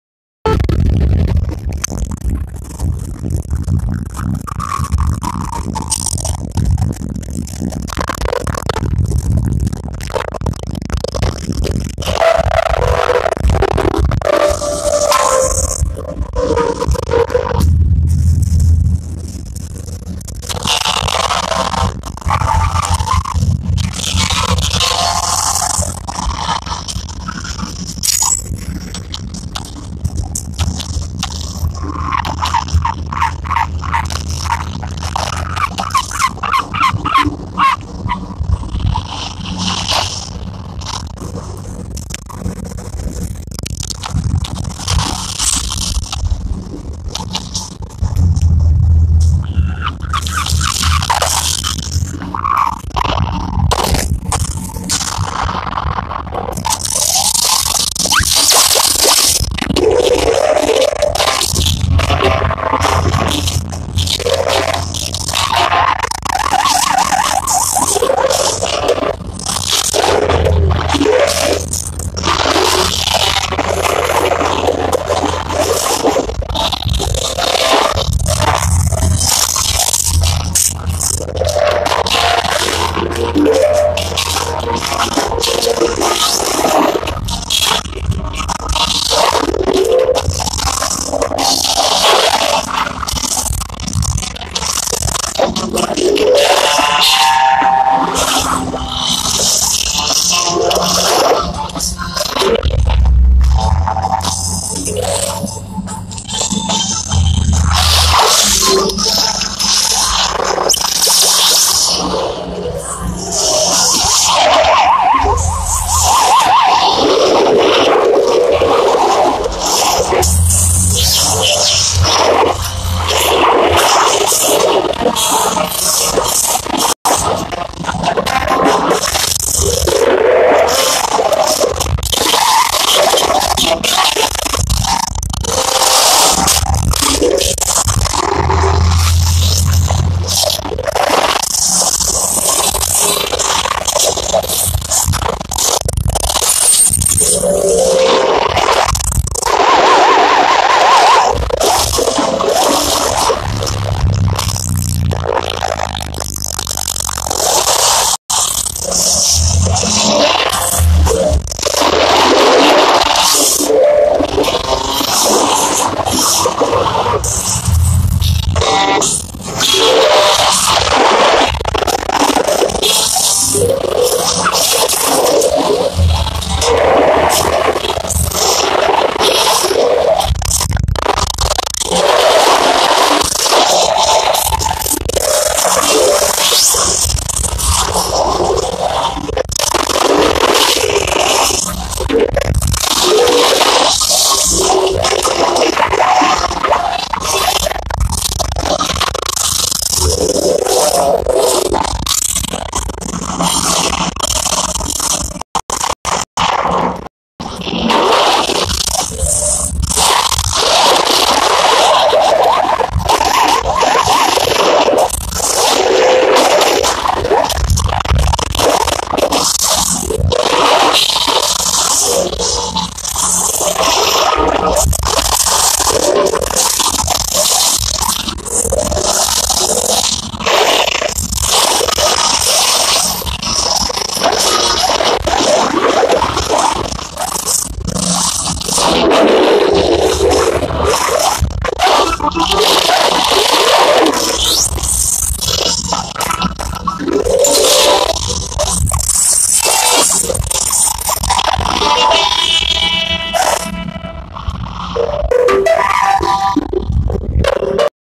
Bowling Bulb
Bowling Bulb: We likes to play bowling because the zombies are the pins